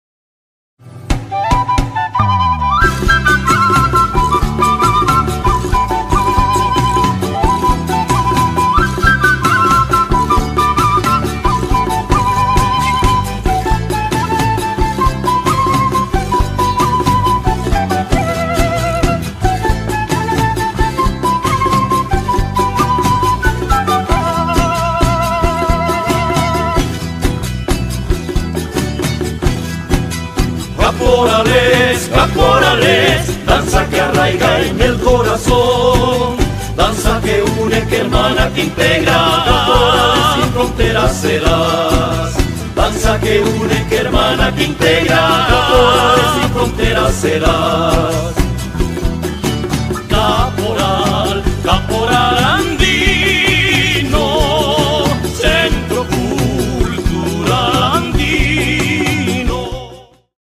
Convocatoria Candelaria 2020
CONVOCATORIA - CANDELARIA Puno 2020

TaytasCaporales te invita a participar en febrero de la Festividad virgen de la Candelaria 2020.